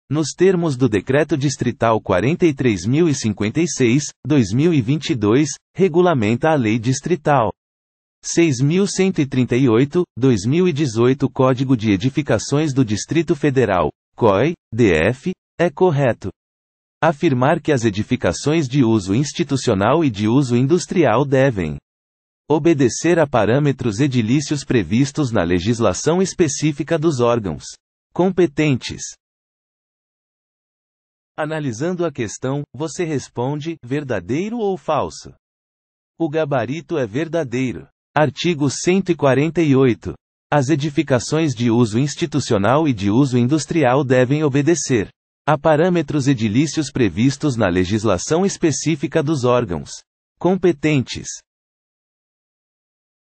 QScon Direito: [Art. 148, Dec Distrital 43.056/2022] Texto de Lei
Art. 148, Dec Distrital 43.056/2022 | Texto de Lei

Nos termos do Decreto Distrital 43.056/2022 (Regulamenta a Lei Distrital. 6.138/2018 - Código de Edificações do Distrito Federal - COE/DF), é correto. afirmar que as edificações de uso institucional e de uso industrial devem. obedecer a parâmetros edilícios previstos na legislação específica dos órgãos. competentes.

Gabarito: Verdadeiro